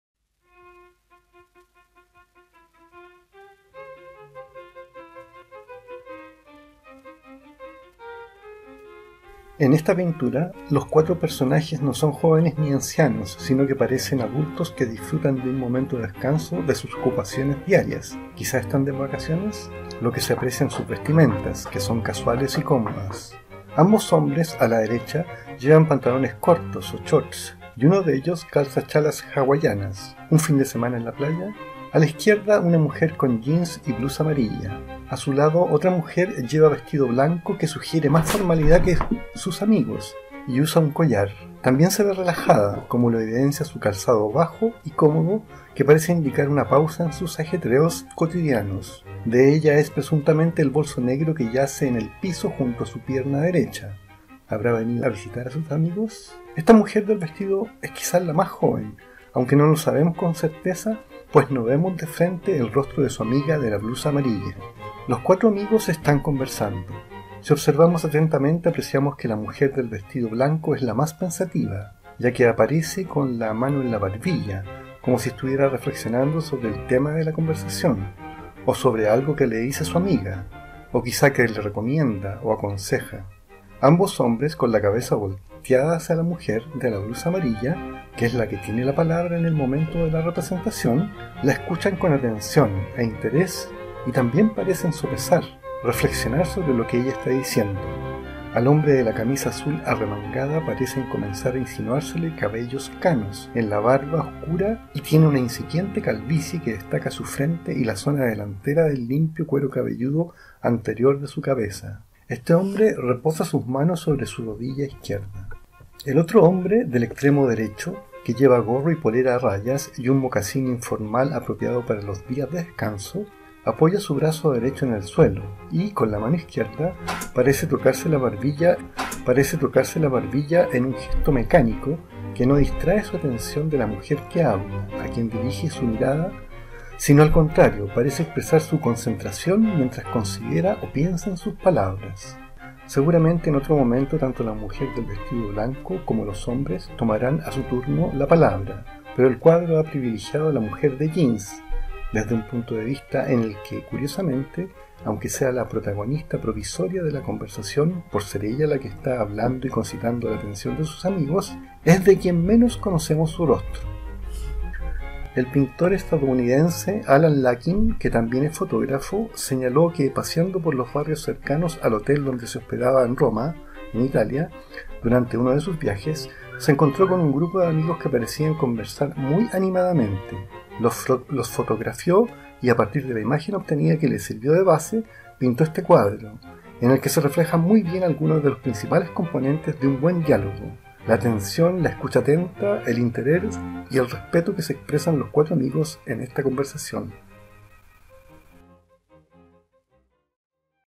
ALAN LAKIN, "LA CONVERSACION", pintura
"LA CONVERSACIÓN" (2014), ALAN LAKIN, pintura

Durante un viaje a Italia, y mientras salía a recorrer el barrio donde se ubica el hotel donde se hospedaba, en Roma, el autor se encontró con un grupo de amigos charlando. Elaboró la pintura a partir de la fotografía que tomó de esas personas, procurando recrear la atmósfera de amistad e interés que se expresaba en la animada conversación.